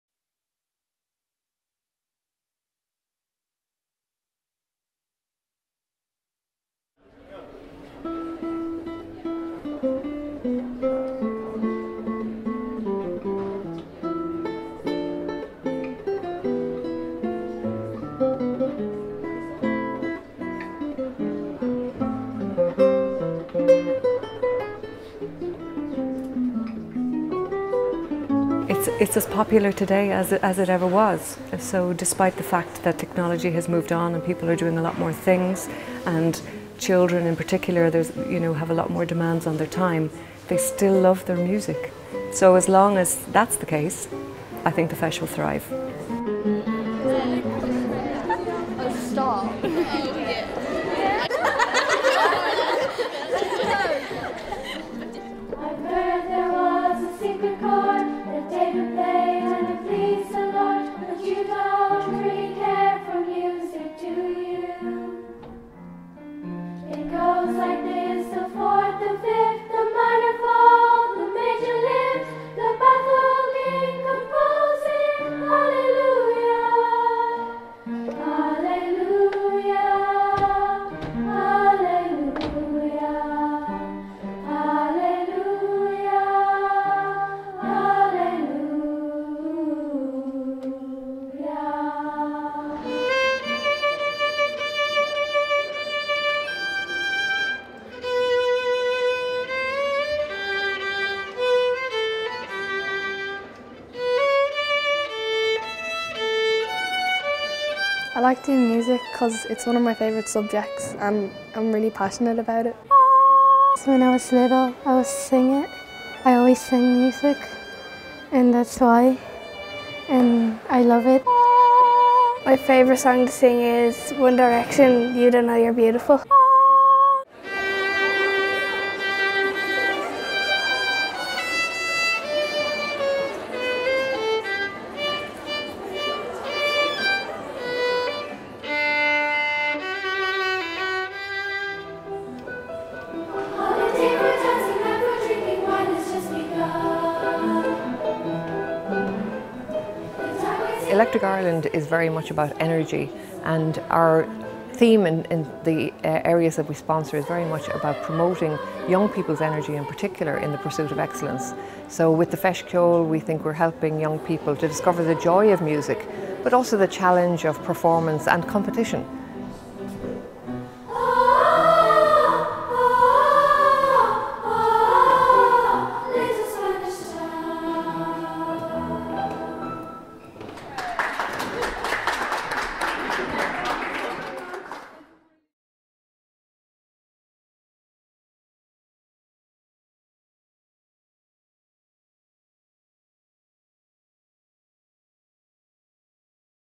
Electric Ireland Feis Ceoil 2013: A Classical Celebration
Electric Ireland Feis Ceoil 2013, March 11-22 @ The RDS, Dublin.

A short film about the launch of the Electric Ireland Feis Ceoil 2013 at the Natural History Museum, Dublin, starring the Rockford Manor Choir, classical guitarist Liam McManus and violinist Juliana Odubajo.